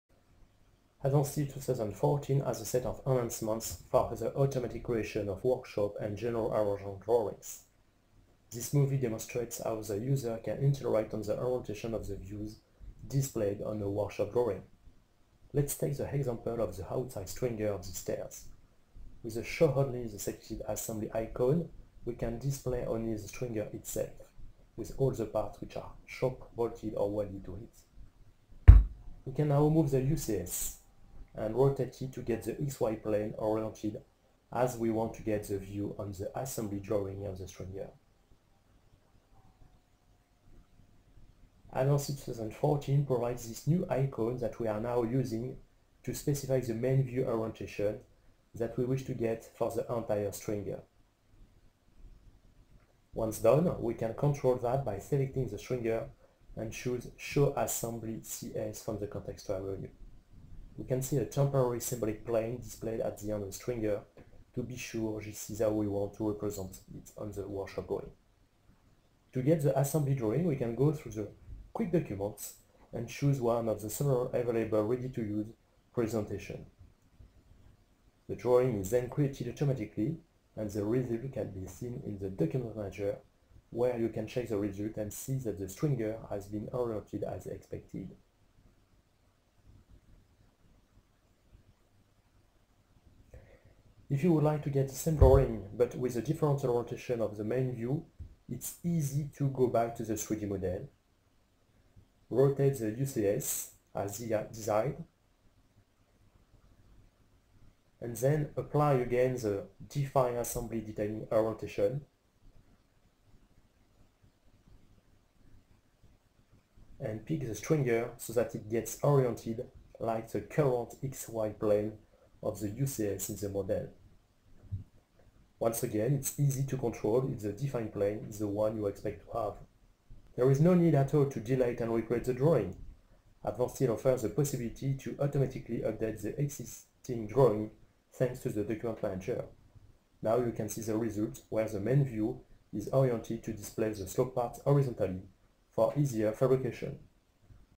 NEW - Advance Steel 2014 - Define detailing orientation
Advance Steel 2014 has a set of enhancements for the automatic creation of workshop and general arrangement drawings.
This movie demonstrates how the user can interact on the orientation of the views displayed on a workshop drawing.